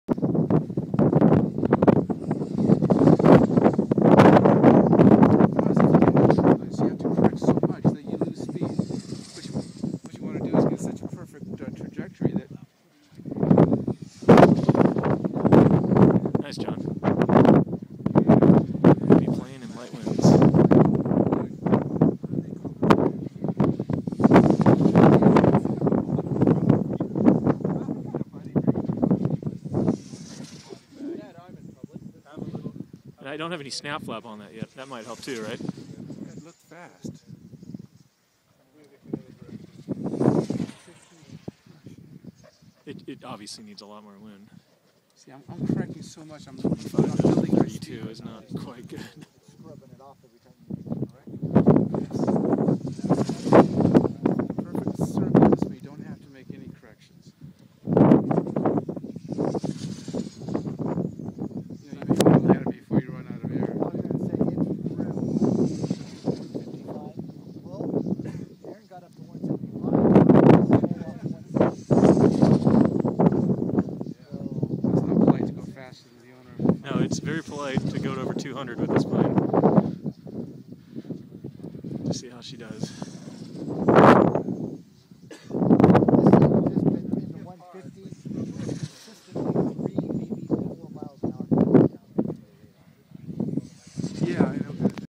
Daisy Cutter 60 flown by John Buxton
194 oz. DC60 flown by John Buxton at Weldon in 25-32mph winds. At 60oz/sf wing loading, the plane handled exceptionally well in the light conditions.